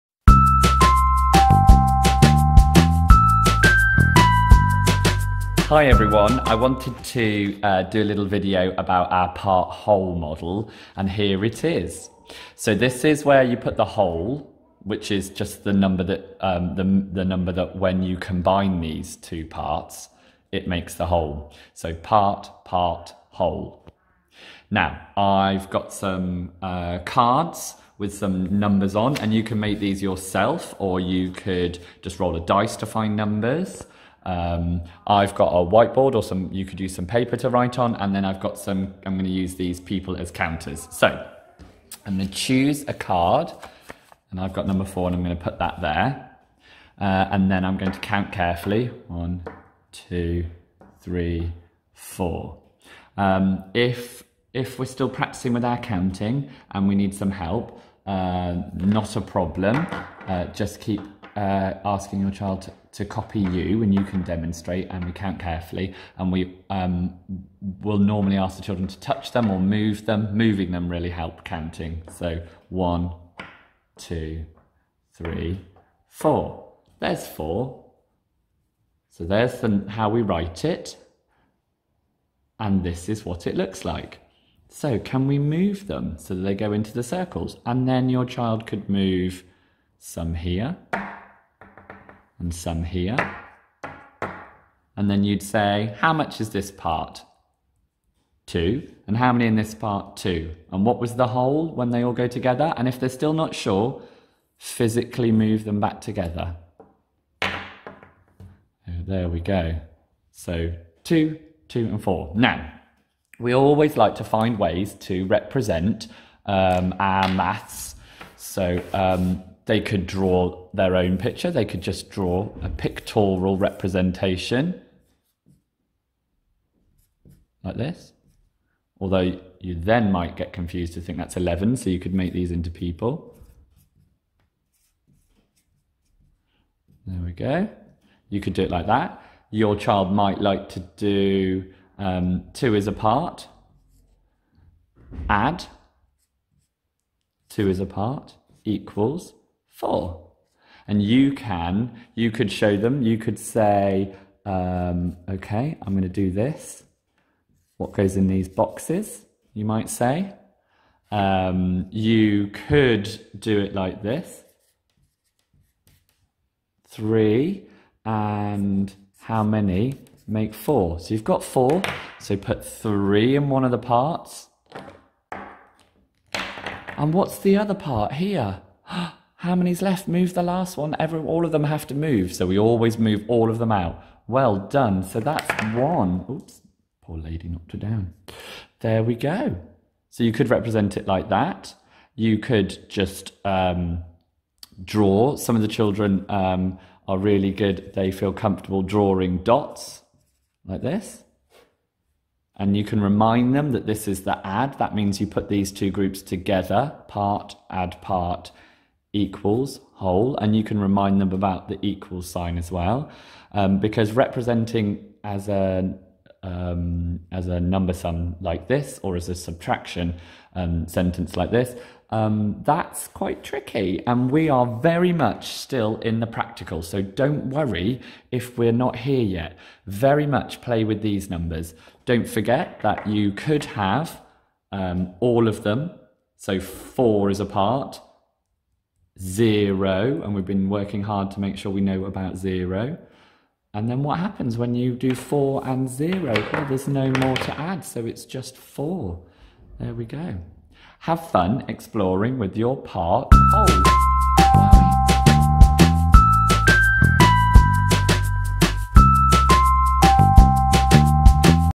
How to use the part whole model
In this video, we explain the basics to using a part whole model. This is a method of finding ways to partition numbers into 2 parts. It's perfect for developing early maths skills!